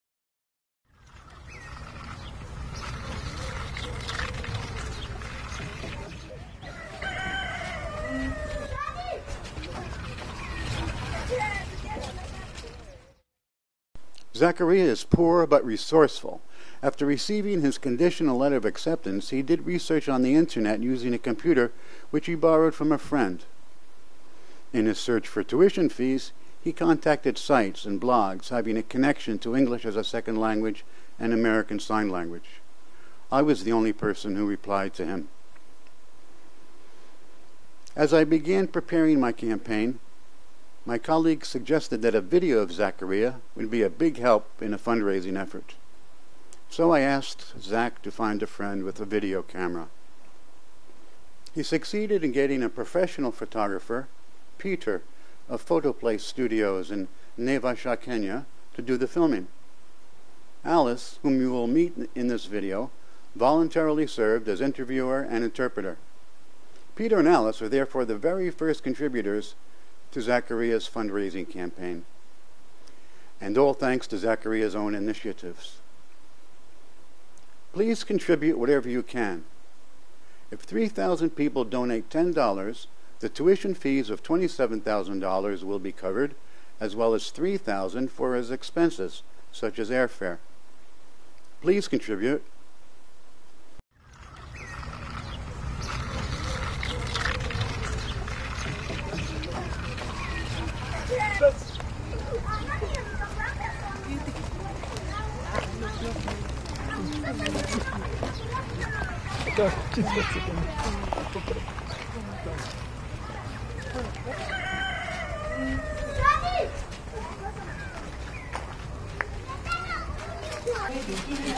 Real English® Fundraiser for the Deaf: Introduction by Michael Marzio (Short Version)
This Fundraiser has been completed. Zach is now attending a school for the deaf in Kenya.